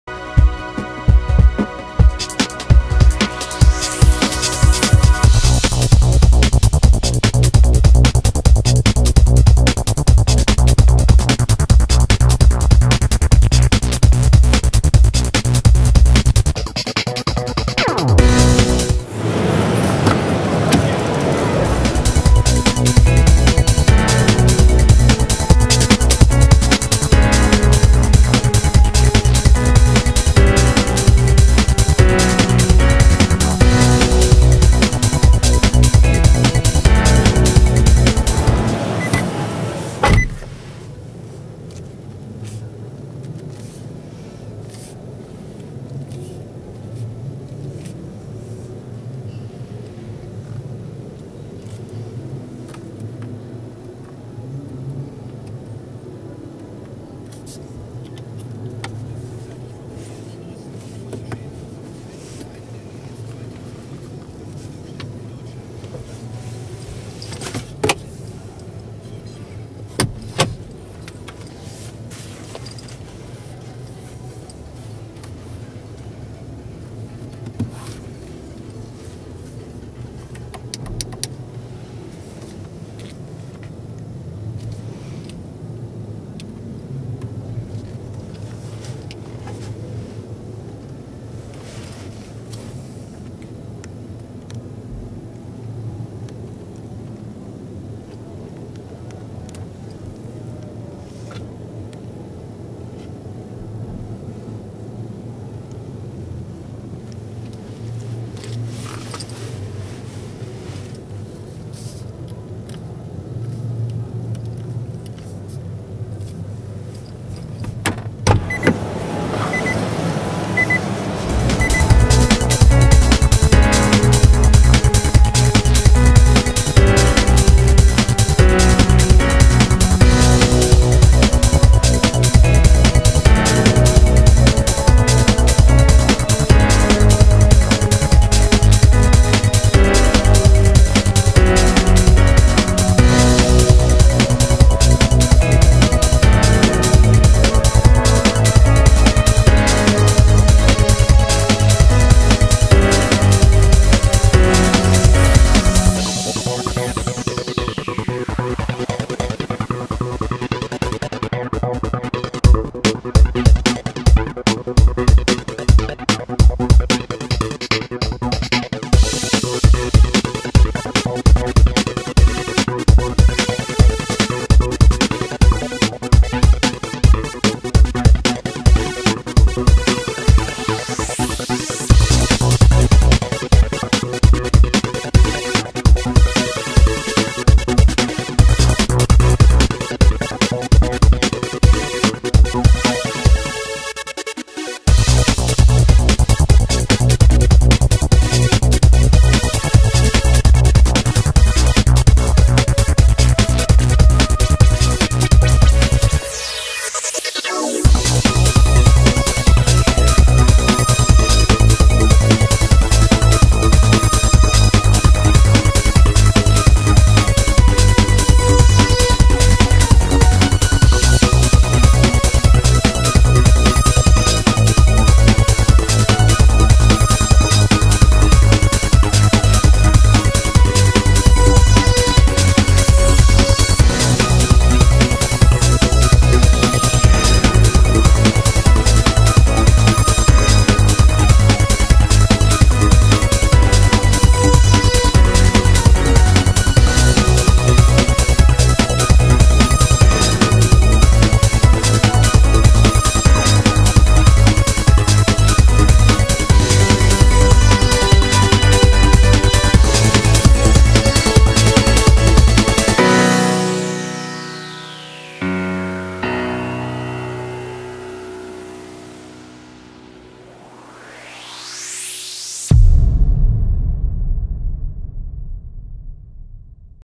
Nissan E-NV200 Evalia (2016) Exterior and Interior
2016 Nissan E-NV200 Evalia car seen from outside and inside. Electric engine. Automatic transmission. 5 doors (5 or 7 seats). Carbon dioxide emissions of 0 g/km. Color is Pearl White. Prices start from 30870 EUR. Shown vehicle costs 32 101 EUR.